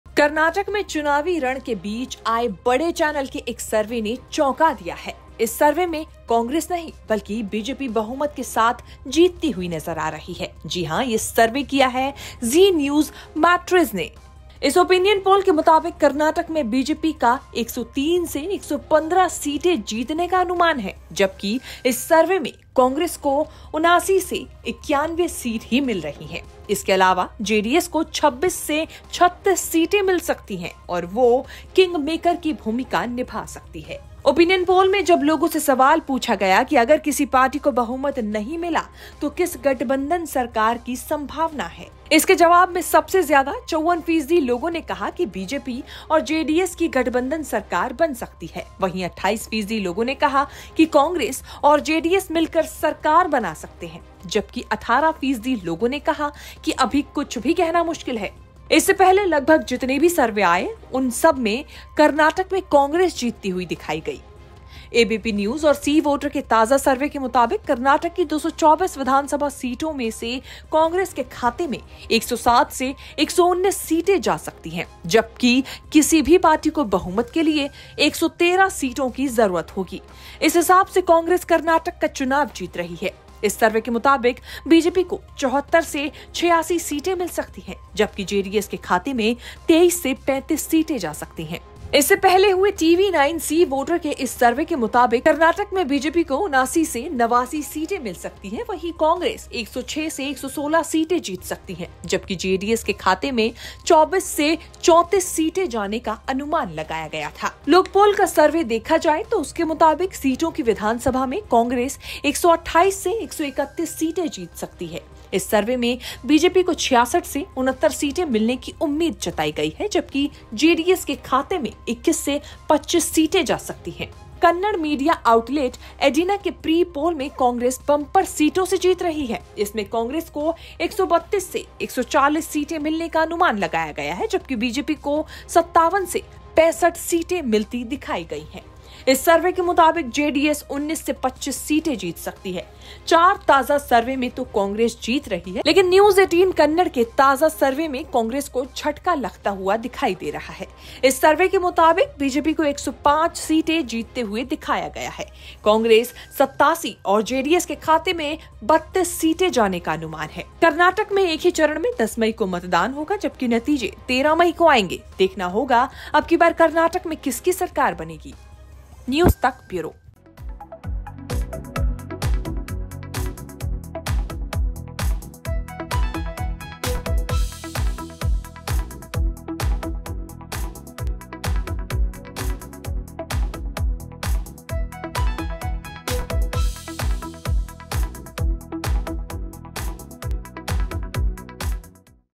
Karnataka का सबसे अलग Opinion Poll, बड़े चैनल के इस सर्वे ने चौंकाया!
कर्नाटक में चुनावी रण के बीच आए बड़े चैनल के एक सर्वे ने चौंका दिया है... इस सर्वे में कांग्रेस नहीं बल्कि बीजेपी बहुमत के साथ जीतती हुई नजर आ रही है... जी हां ये सर्वे किया है जी न्यूज़ MATRIZE ने... इस ओपिनियन पोल के मुताबिक कर्नाटक में बीजेपी का 103 से 115 सीटें जीतने का अनुमान है... जबकि इस सर्वे में कांग्रेस को 79 से 91सीटें ही मिल रही हैं... इसके अलावा जेडीएस को 26 से 36 सीटें मिल सकती हैं और वो किंगमेकर की भूमिका निभा सकती है...